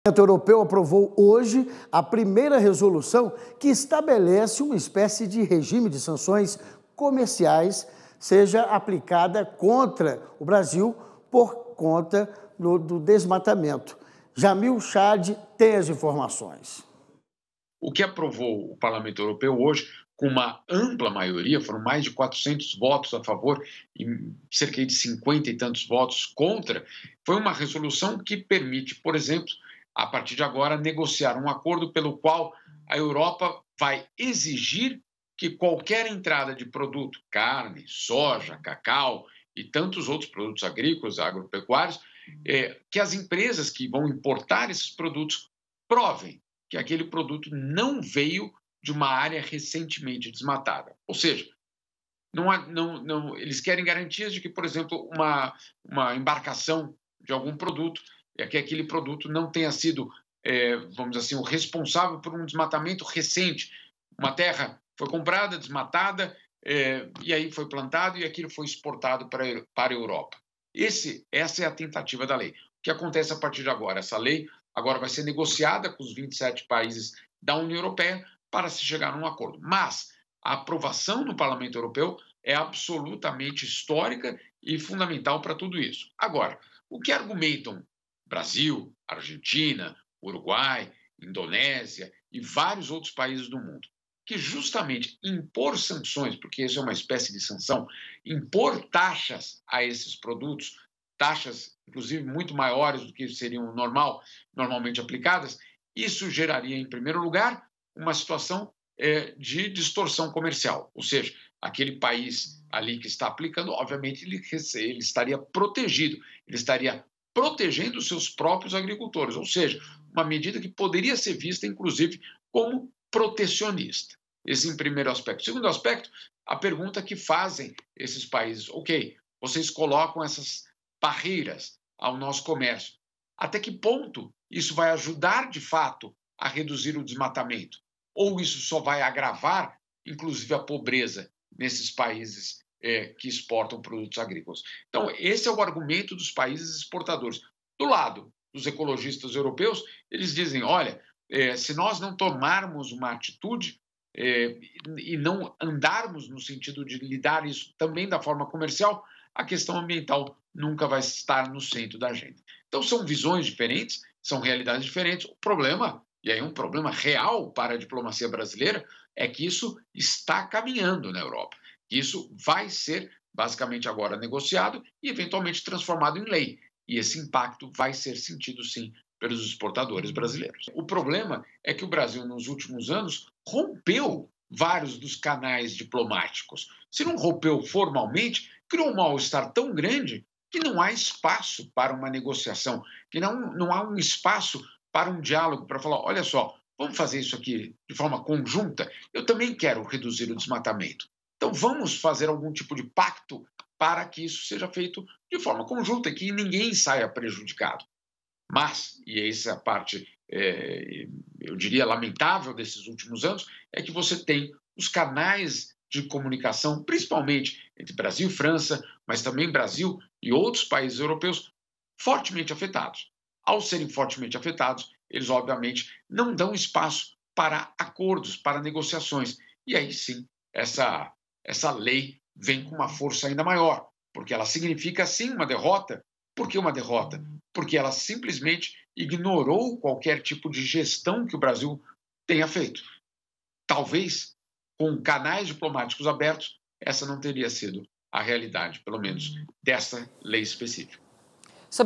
Parlamento Europeu aprova regime de seções ao Brasil
O Parlamento Europeu aprovou, hoje, a primeira resolução que estabelece uma espécie de regime de sanções comerciais que seja aplicada contra o Brasil por conta do desmatamento.Jamil Chade tem as informações. I AgroMais